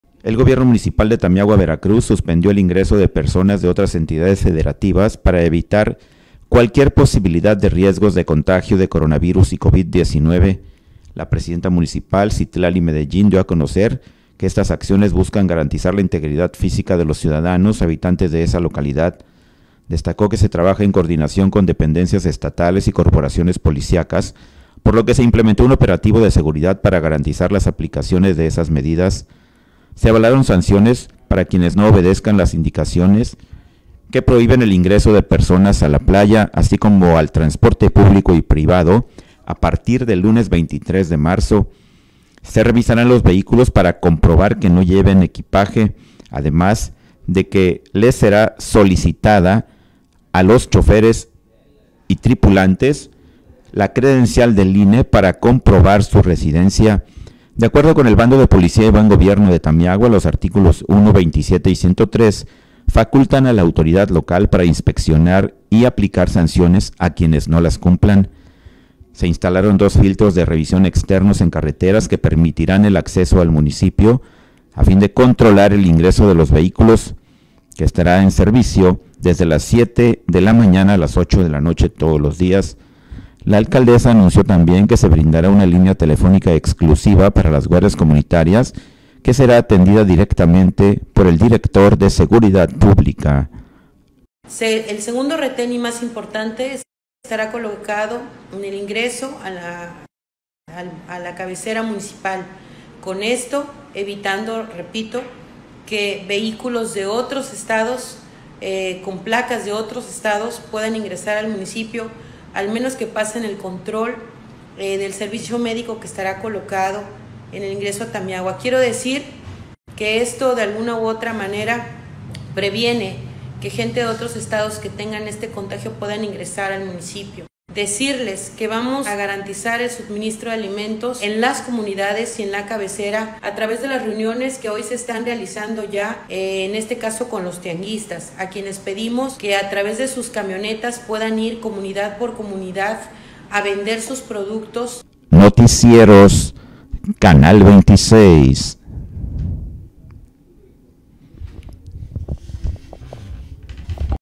Suspenden Ingreso de Personas y de Vehículos de Otros Estados a Tamiahua Veracruz
El gobierno municipal de Tamiahua Veracruz, suspendió el ingreso de personas de otras entidades federativas para evitar cualquier posibilidad de riesgos de contagio de Coronavirus y Covid 19.

La presidenta municipal, Citlali Medellín, dio a conocer que estas acciones buscan garantizar la integridad física de los ciudadanos habitantes de esa localidad.

Destacó que se trabaja en coordinación con dependencias estatales y corporaciones policiacas, por lo que se implementó un operativo de seguridad para garantizar la aplicación de tal medida.

Se avalaron sanciones para quienes no obedezcan las indicaciones tales como el ingreso de personas a la playa, así como al transporte público y privado; a partir del lunes 23 de marzo se implementarán nuevas estrategias.

Se revisarán los vehículos para comprobar que no lleven equipaje, además de que se les pedirá una su INE para comprobar su residencia.

De acuerdo con el Bando de Policía y Gobierno de Tamiahua, los artículos 1, 27 y 103 facultan al Ayuntamiento para inspeccionar y aplicar sanciones a quien no las cumplan.

Se instalaron dos filtros de revisión externos en carreteras, que permitan el acceso al municipio para controlar el ingreso de los vehículos, que estará en servicio desde las 7:00am a 8:00 pm todos los días.

La alcaldesa también anunció que se brindará una línea telefónica exclusiva para las guardias comunitarias que estará atendida directamente por el Director de Seguridad Pública Yoani Marcelo Martínez, esto para brindar la atención a todos los reportes por parte de los coordinadores ruta.